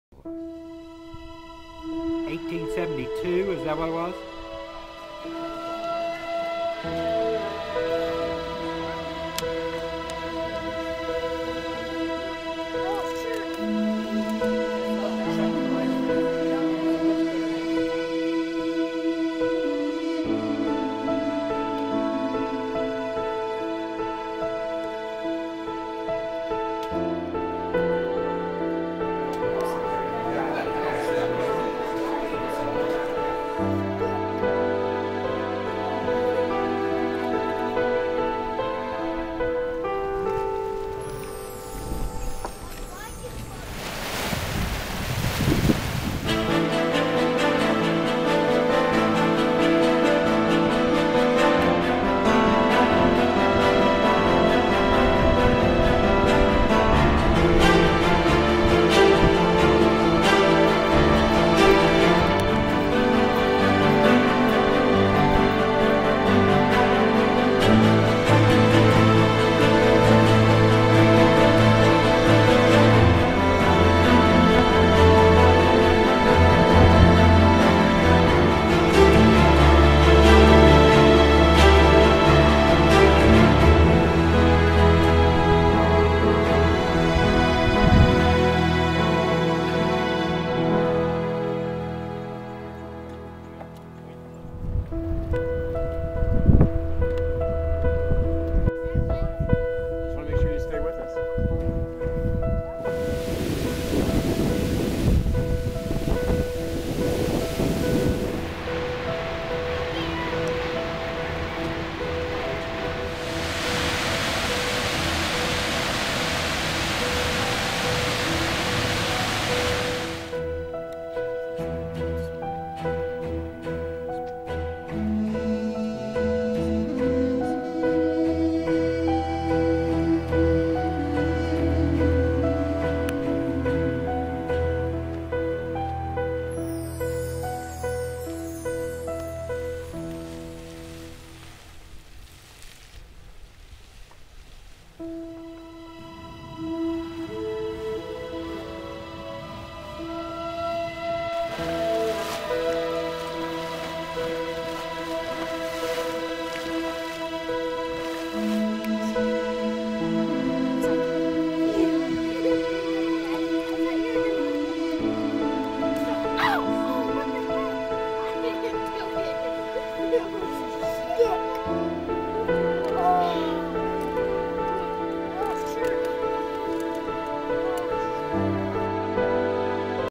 Yellowstone
This is footage from the second time my brother Jerry and I visited Yellowstone.  It is a beautiful surreal place on this earth in the state of Wyoming.  Take time make a plan to see it for yourself.  A must for your bucket list. check!